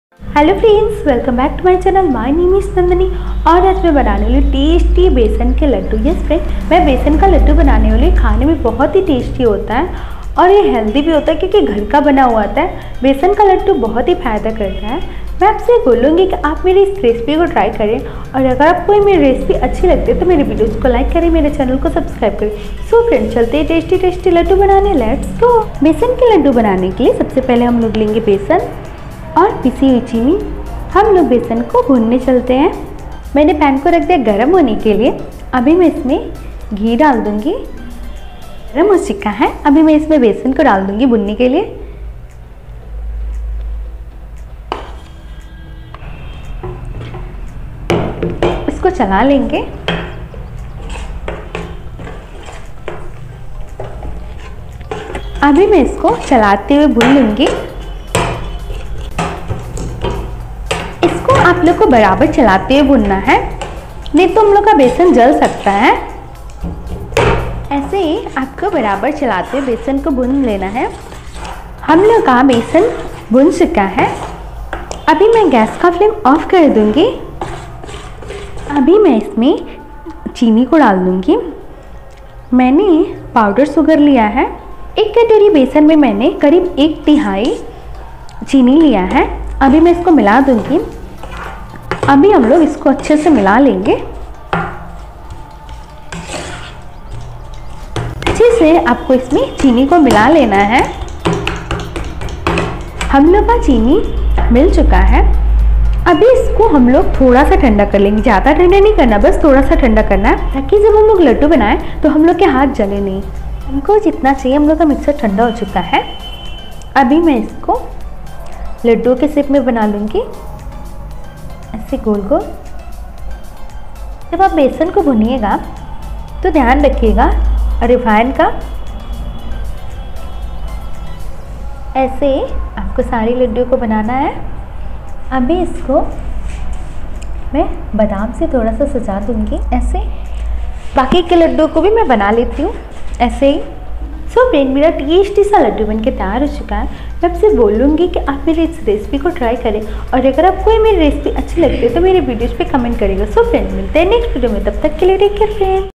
लड्डू तो बहुत खाये होंगे पर एक बार बेसन के स्वादिष्ट लड्डू की रेसिपी जरूर देखे| Besan laddoo recipe
besan ke laddu ki tasty  recipe ek bar jarur banaye.